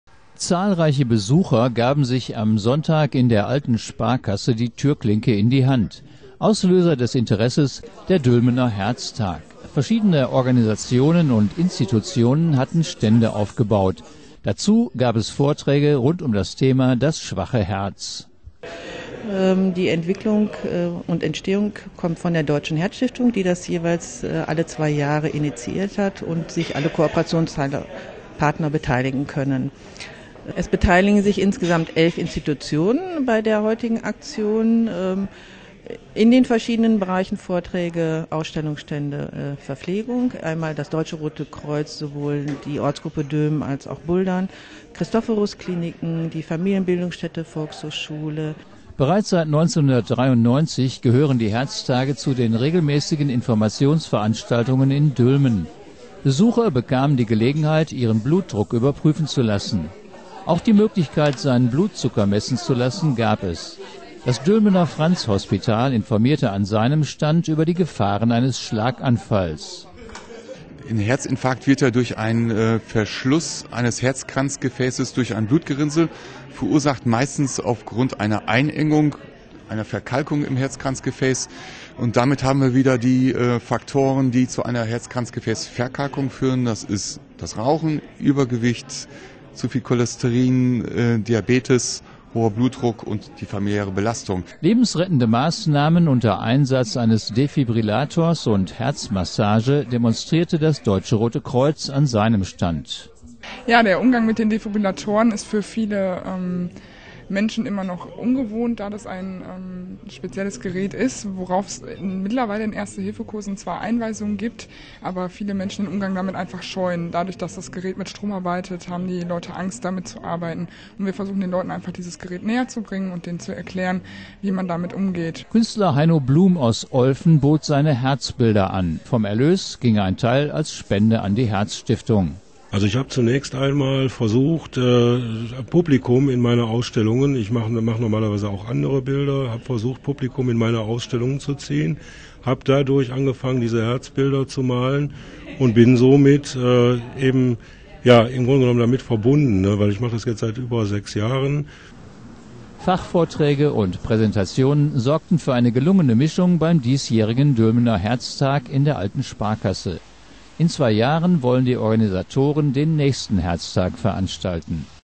Herztag in Dülmen
Zahlreiche Besucher gaben sich am Sonntag in der Alten Sparkasse die Türklinke in die Hand. Auslöser  des Interesses: Der Dülmener Herztag. Verschiedene Organisationen und Institutionen hatten Stände aufgebaut. Dazu gab es Vorträge rund um das Thema "Das schwache Herz".